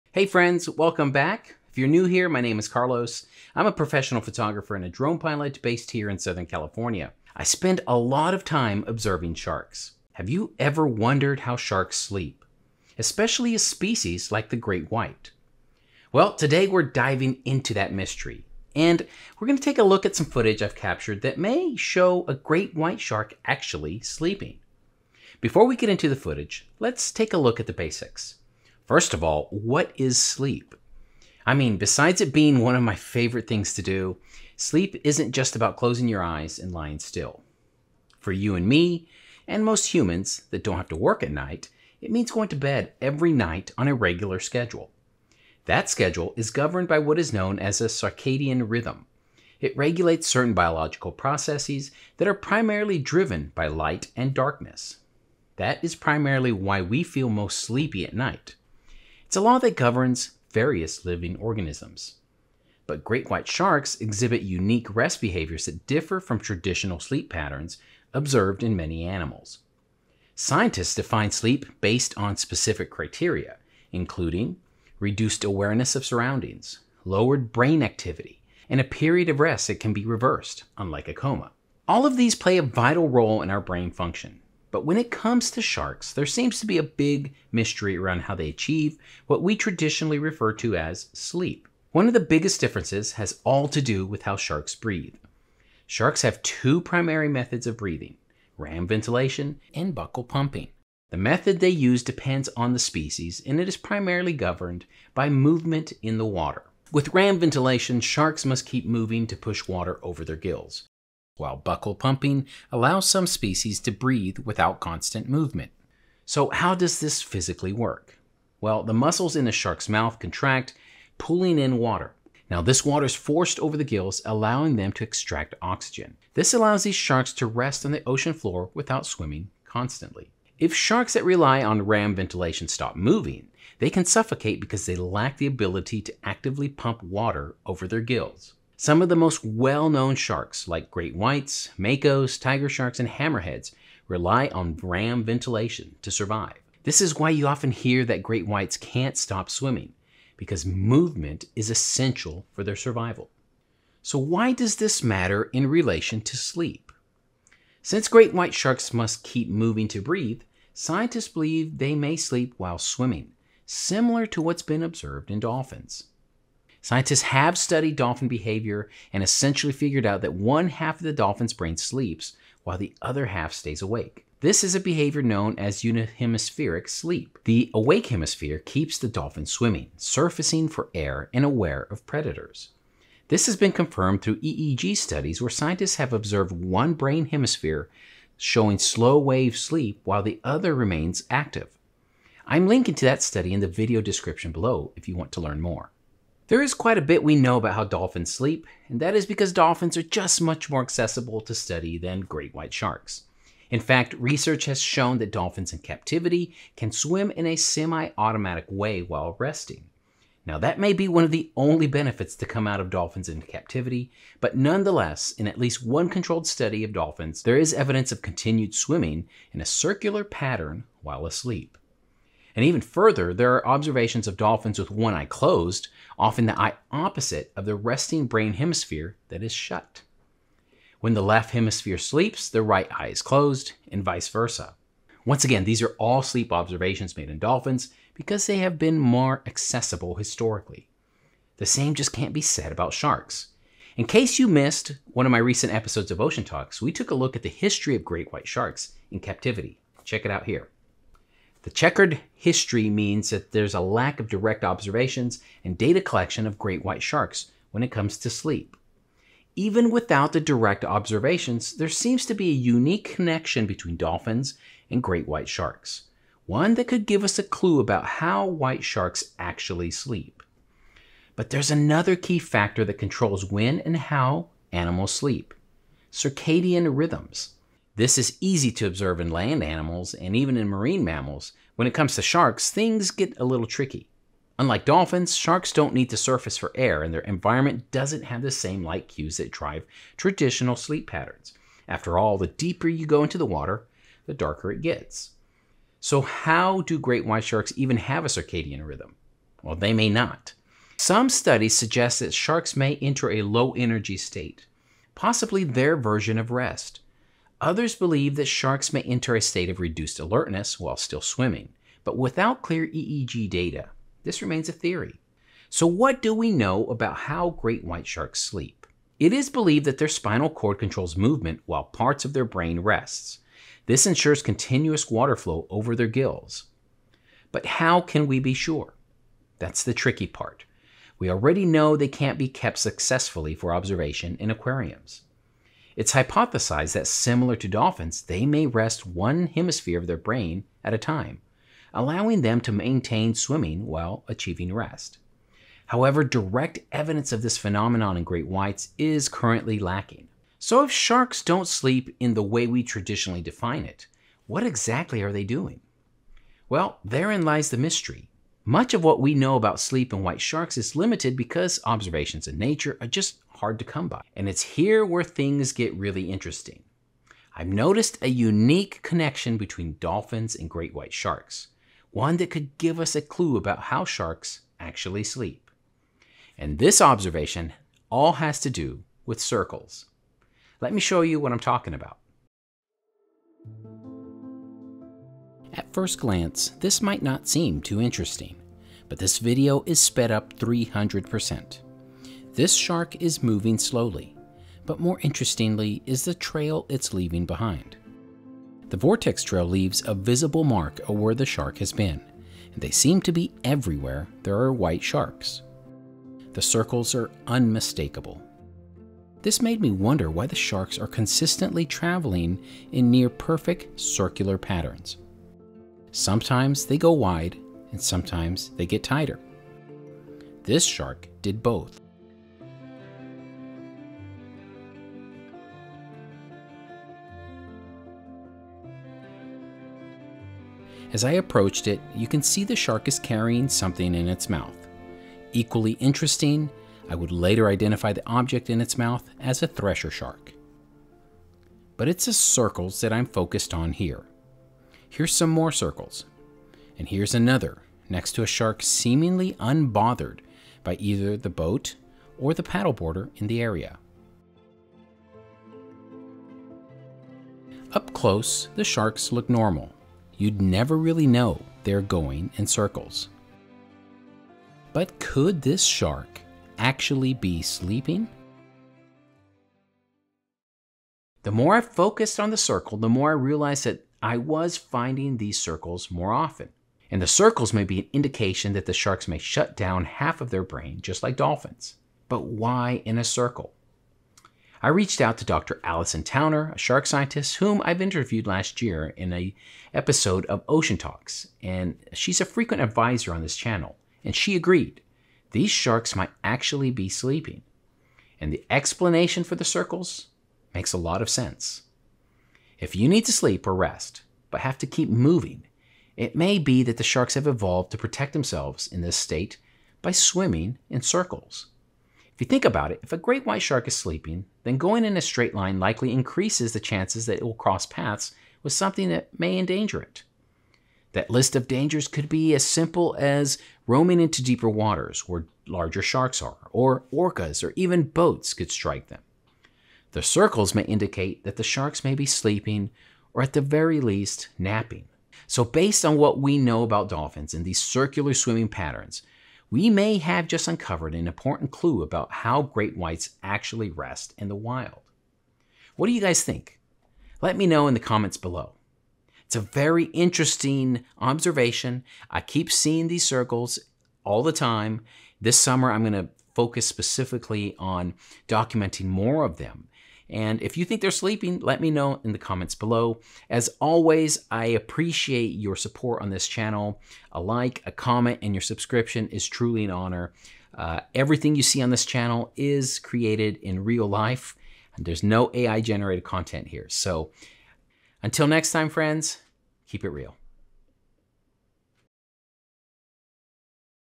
This Great White Shark Might Be Sleeping
Have you ever wondered how great white sharks sleep—if they sleep at all? In this episode, I dive into one of the ocean’s greatest mysteries using science, expert insight, and my own drone footage that may reveal never-before-seen sleeping behavior in white sharks. From how sharks breathe to the concept of unihemispheric sleep and circadian rhythms, we explore how these apex predators might rest while on the move. And what do circular swimming patterns have to do with it?

Your support is greatly appreciated on this channel.

Or join my YouTube Crew or Explorer Membership.
CHAPTERS
00:00:30- What is Sleep?
00:01:35- How Do Sharks Breath?
00:01:35- Let's talk about how Dolphins Breath
00:06:20- Shark Circles Segment. Are these white sharks sleeping?
CITATIONS: Further Research
Dolphin Sleep Study
All music has been licensed for use.